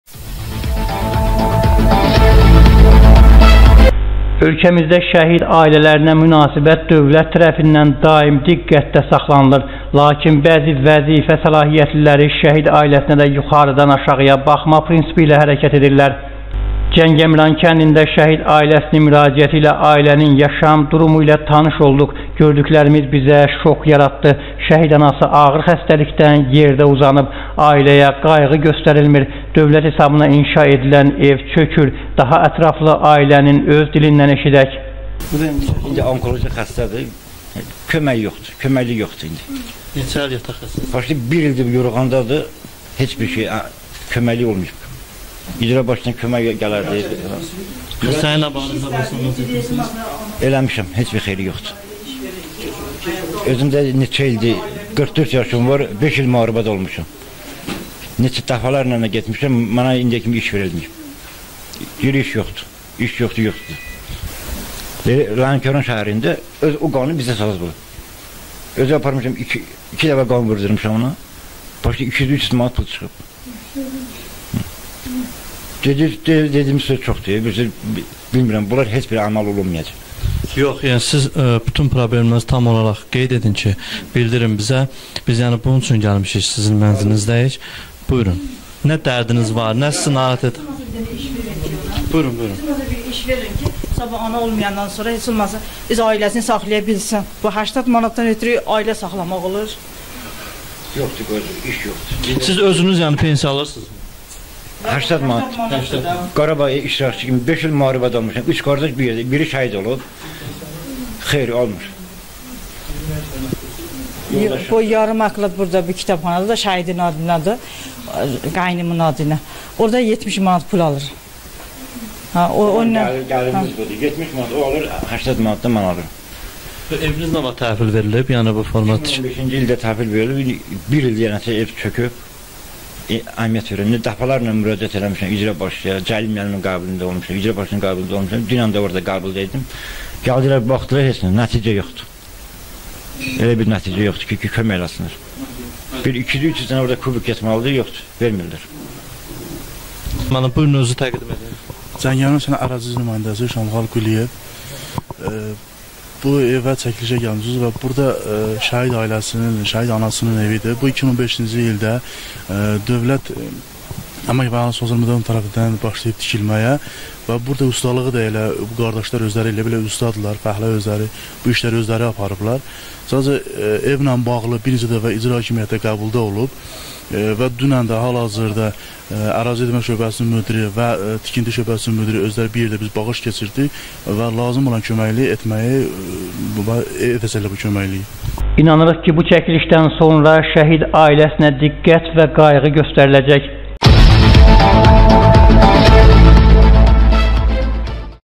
LERİKDƏ ŞƏHİD AİLƏSİNİN FƏRYADINI EŞİDƏN YOXDUR-SƏBƏB?
QARŞILAŞDIĞINIZ PROBLEMLƏRİ ÇƏKİB BİZƏ GÖNDƏRƏ BİLƏRSİNİZ.